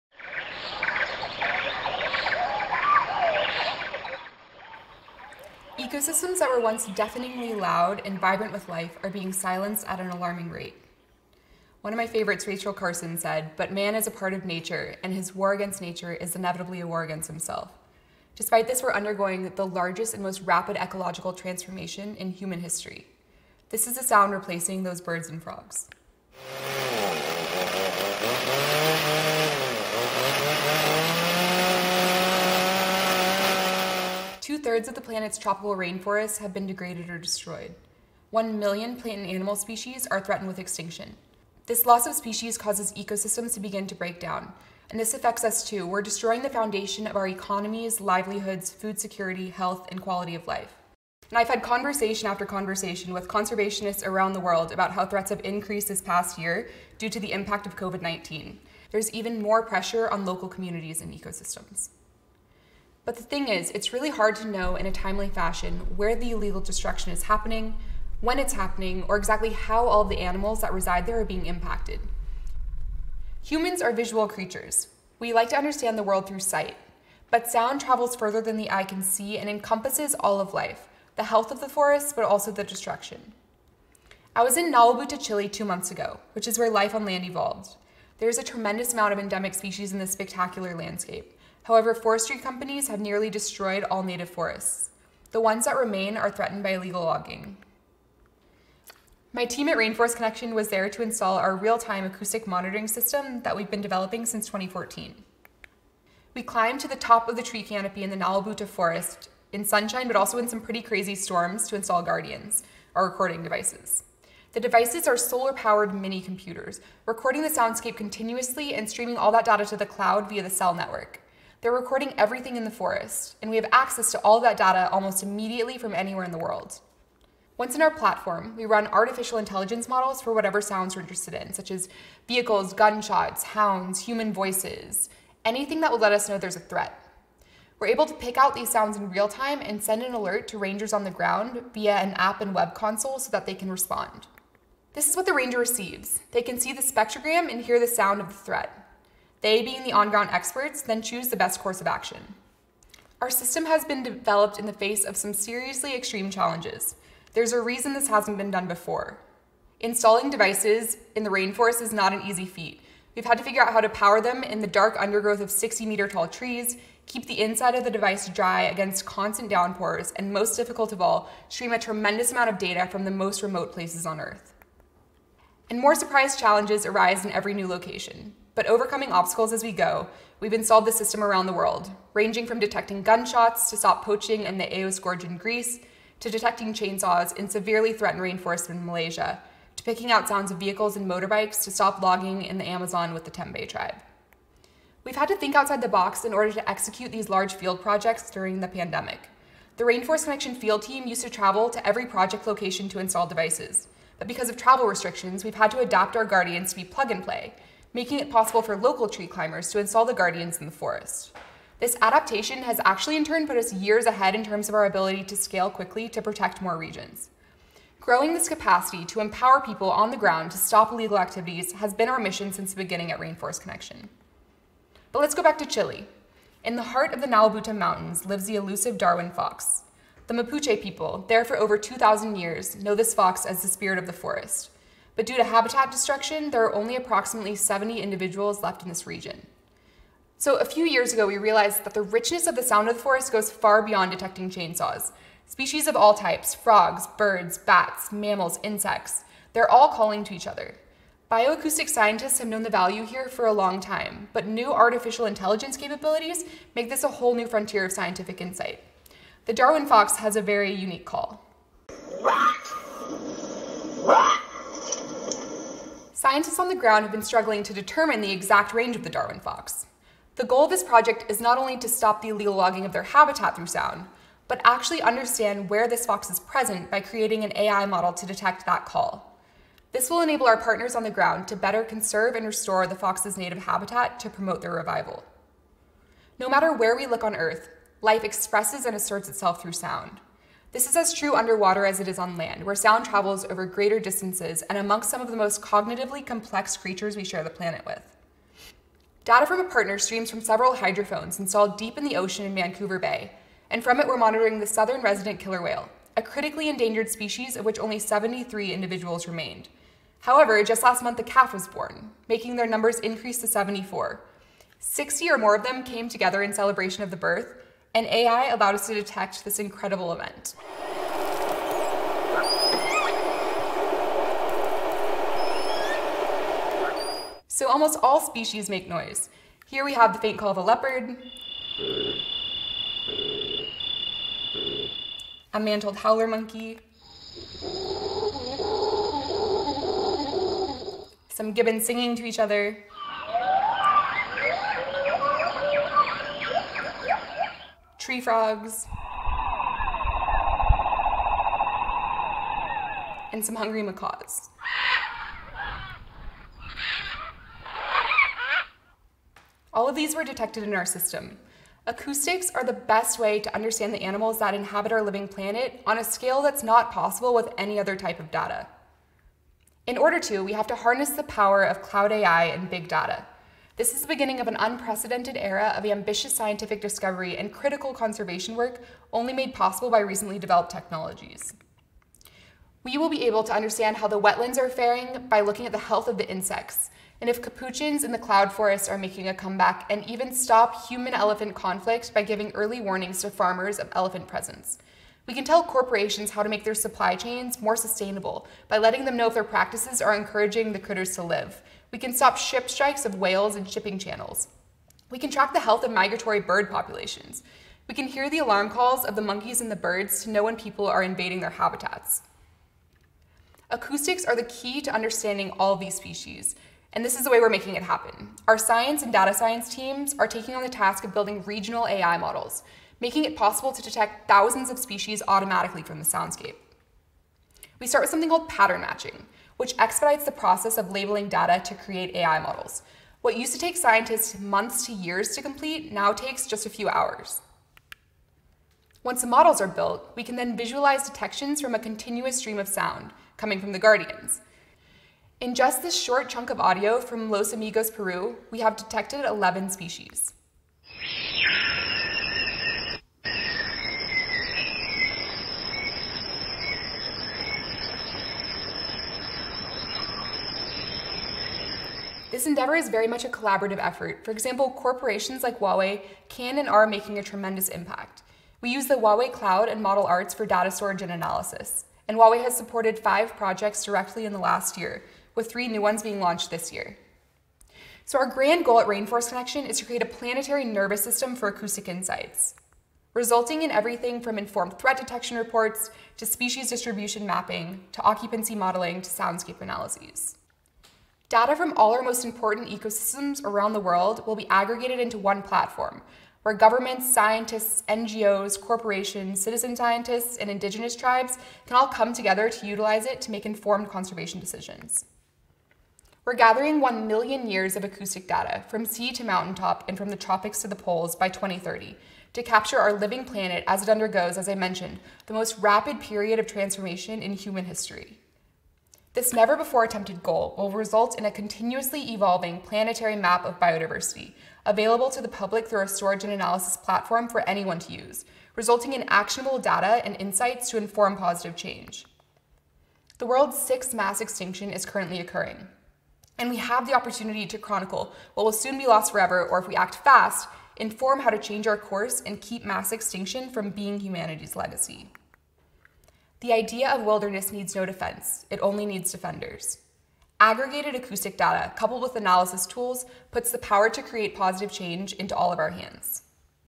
TEDx Driven by Pressure: Saving the Forest One Sound at a Time
The forest isn’t buzzing with life but throbbing with threatening sounds — sounds that can wipe out many faunas and floras. Vibrant ecosystems are being silenced at an alarming rate. Rainforest Connection Director of International Expansion Chrissy Durkin and her team are putting a stop to the destruction with the help of tech. Here’s how.